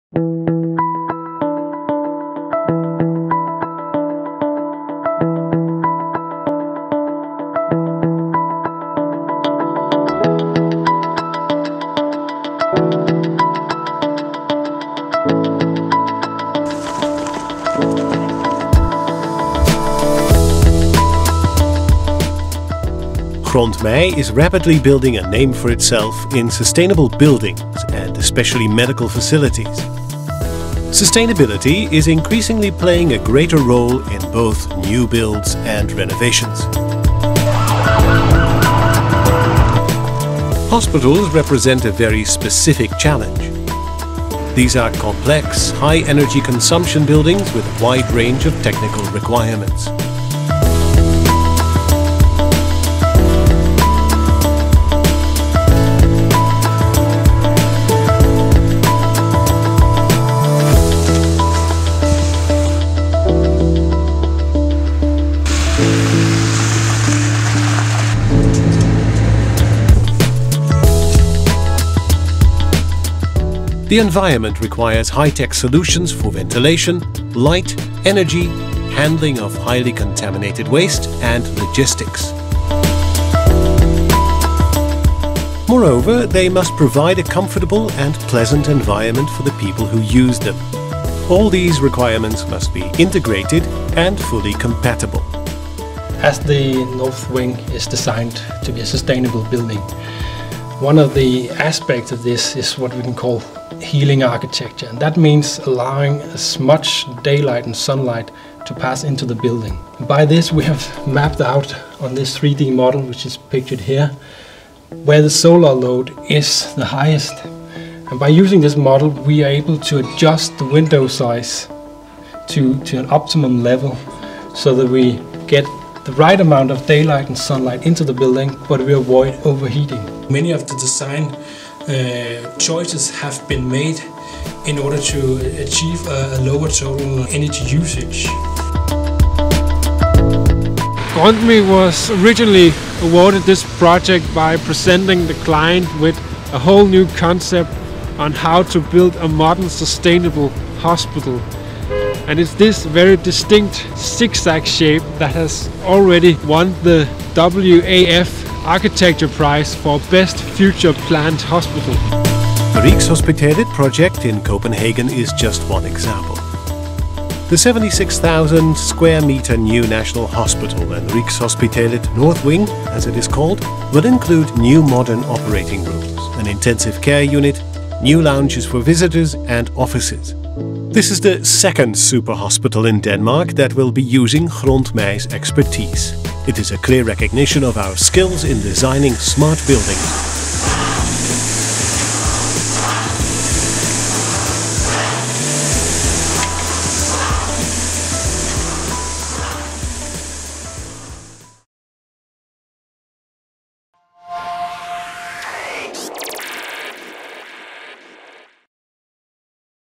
Sustainable Buildings - Healthy development
The award of the Rigshospitalet project in Copenhagen builds on our growing leadership position in Sustainable Buildings, especially medical facilities, around Europe. Sustainability is increasingly playing a greater role in both new builds and renovations. Hospitals represent a very specific challenge. These are complex, high-energy consumption buildings with a wide range of technical requirements.

See the movie of Jonas Vendel Jensen - Grontmij consultant in Denmark.